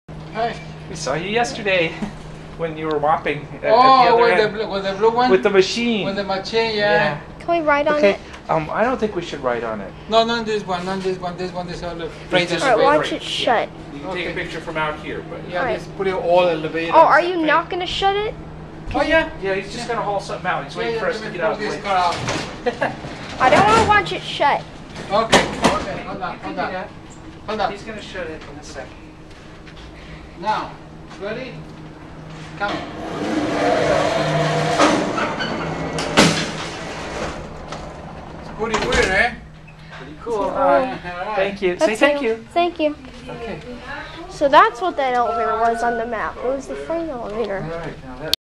A Look on 1 at a Vintage Montgomery Freight Elevator at Dominican Hospital in Santa Cruz, CA
This is a hydraulic elevator.

This video was filmed on September 30. 2011.

EDIT: I was told by an Otis technician that this is an Armor elevator. He probably meant that this elevator had an Armor controller.